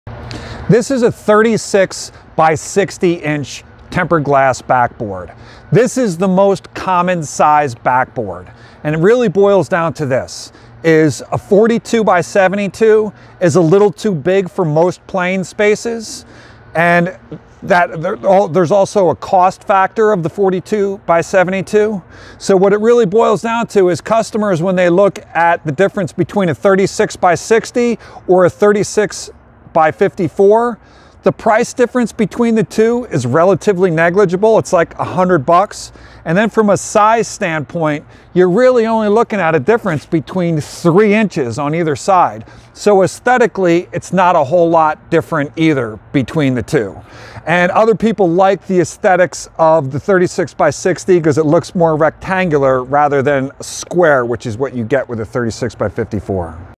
Basketball Hoop Buying I 36"X60" Backboard I
36"X60" Backboard-

This is the most popular size as it falls into the middle range.  Most people don't want to go big but they're willing to cough up a couple bucks to upgrade over the 36"X54"

Other factors:

1. Price difference between 36"X60 and 36"X54" is negligible so why not go with the bigger backboard?
2. It's not that much bigger.  It's only 3" more on either side relative to the 36"X54".
3. Some people prefer the more rectangular look to the more square look of the smaller backboard.